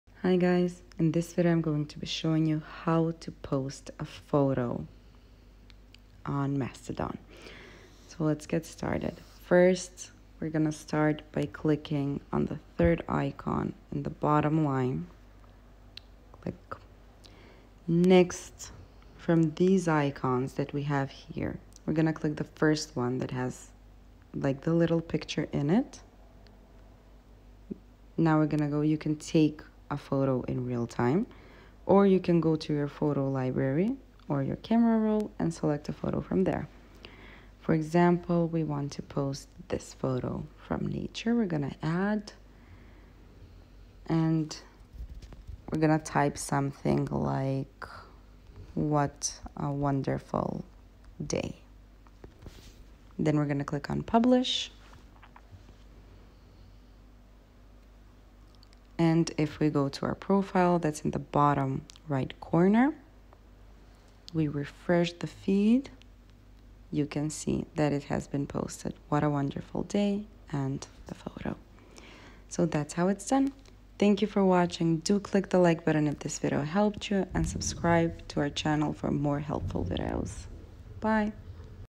How To Post A Post On Mastodon - Quick And Easy!
Learn How To Post A Post On Mastodon. It is really easy to do and learn to do it in just a few minutes by following this super helpful tutorial to get a better understanding of everything.